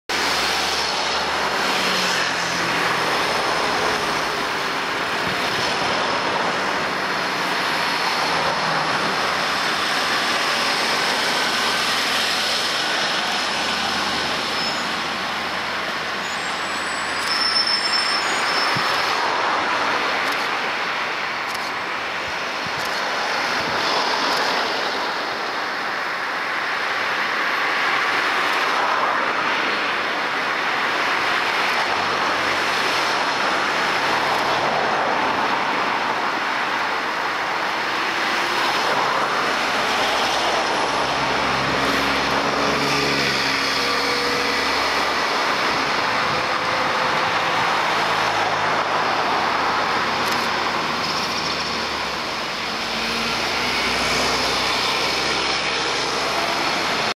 Cycle lanes on Huntingdon Road
Cyclists on Huntingdon Road should benefit from segregation from motor vehicles and the junctions need improving. Respond to the consultation